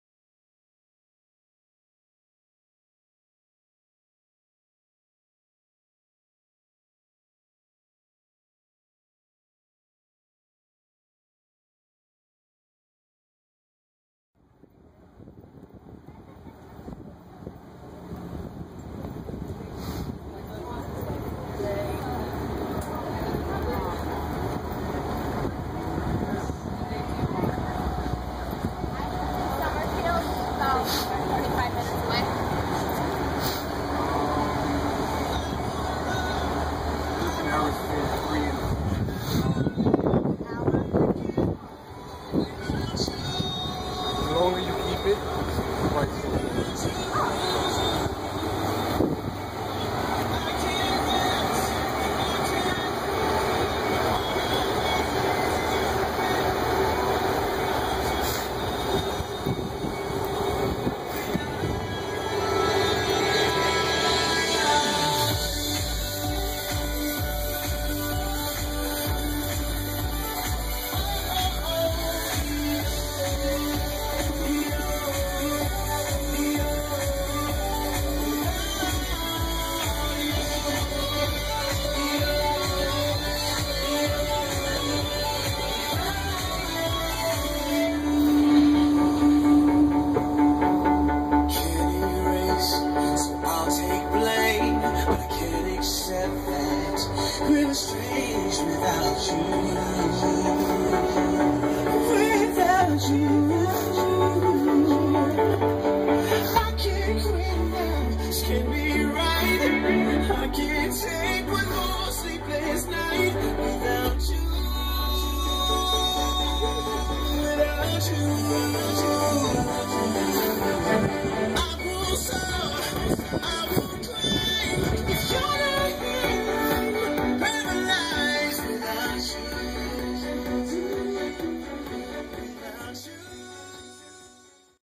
CEO Limos H2 Hummer Limo at Bridal Show in Ocala BIGGER THAN A PARTY BUS!!!!
I saw this HUGE Hummer Limo at a Bridal Show at Ocala National Golf Club.